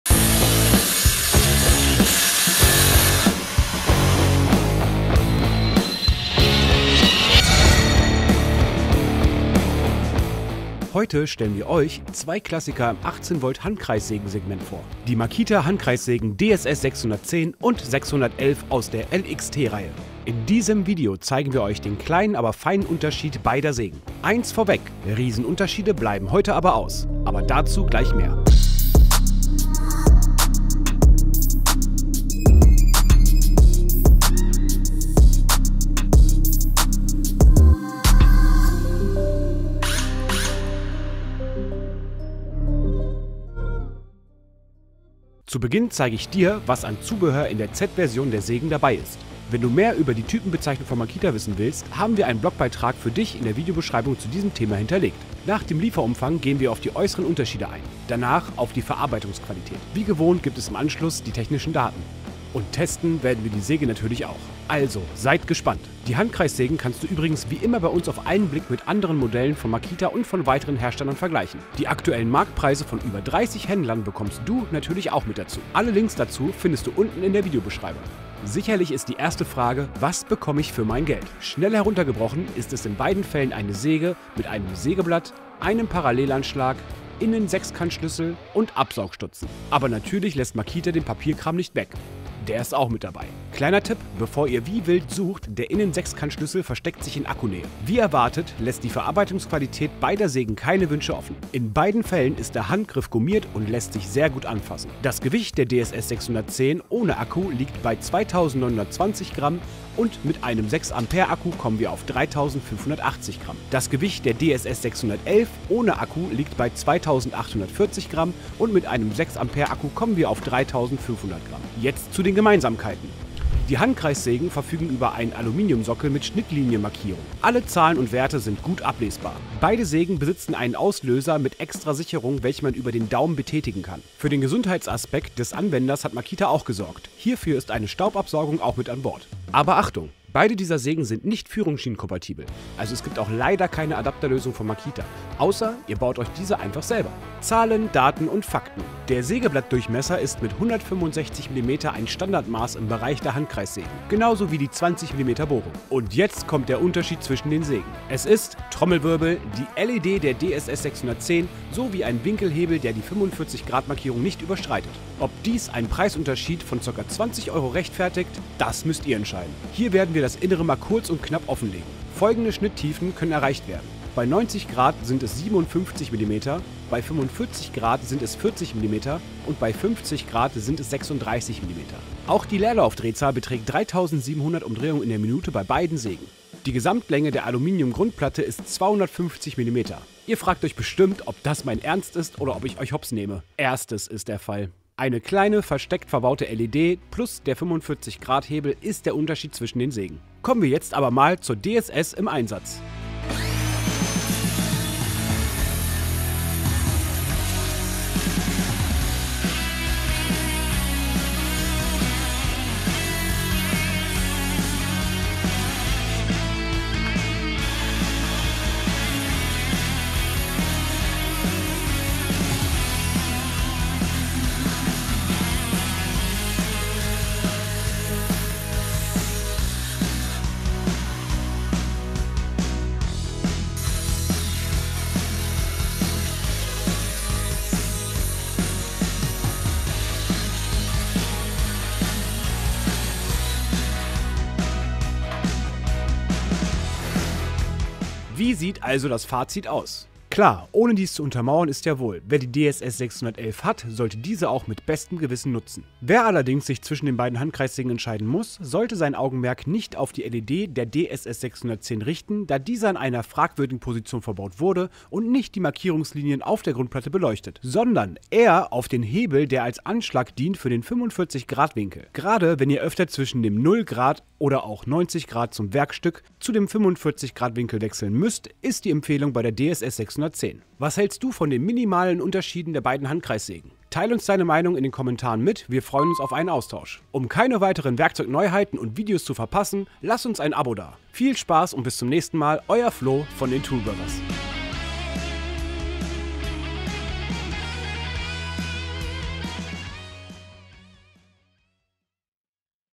🤔 FÜR WELCHE SOLL ICH MICH ENTSCHEIDEN? MAKITA DSS610 vs DSS611 HANDKREISSÄGEN VERGLEICH & TEST
Die DSS 610 und DSS 611 ähneln sich, wie ein Ei dem Anderen. Oder etwa doch nicht? Denn trotz der großen Ähnlichkeit, herrscht zwischen den beiden Geräten auch in der Solo-Version ein Preisunterschied. Was ist hier also nicht richtig? Wir schauen uns die beiden Akku Handkreissägen-Klassiker genauer an und zeigen dir, wo die Unterschiede liegen. Gerade, wenn Du vor der Entscheidung zwischen den beiden Modellen stehst, helfen wir dir hier weiter! Viel Spaß mit diesem kurzen Tool-Check.

So bleibst Du immer Up-To-Date, vergisst kein GEWINNSPIEL und kein Video. Werde Teil der Toolbrothers-Fam!

Inhalt:

00:00 Intro
01:19 Lieferumfang
01:38 Technische Details
02:37 Unterschiede
03:23 Tests
04:09 Fazit